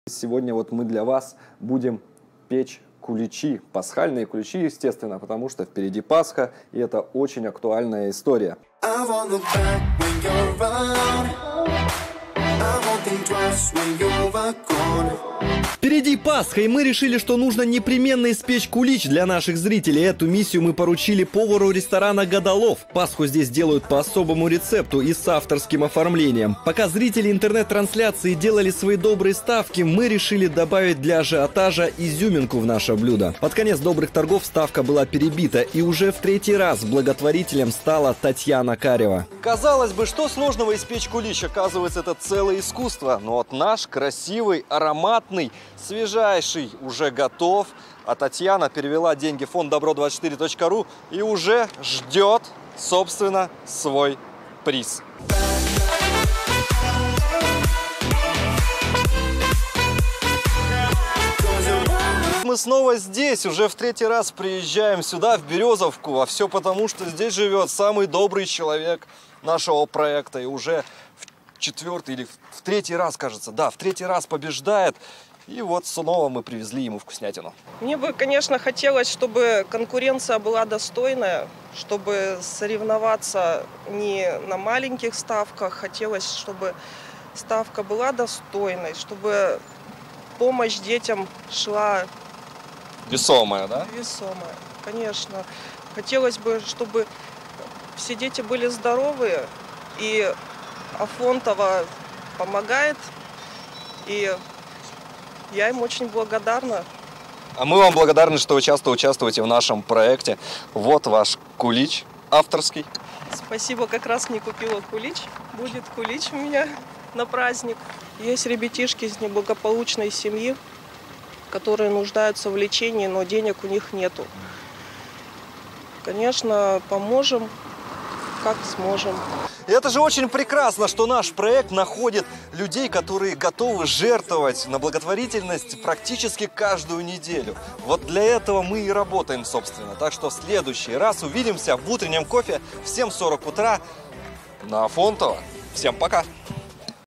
Ставка на добро: пасхальный кулич
Готовимся к Пасхе вместе с нами. Накануне наш благотворительный проект заглянул в гости в ресторан «Гадалов»: там вместе с поваром мы готовили кулич по авторскому рецепту​. За приготовлением блюда следили зрители онлайн-трансляции и делали свои добрые ставки. Как прошел аукцион и кто в этот раз стал благотворителем — расскажет Антон Шевчук.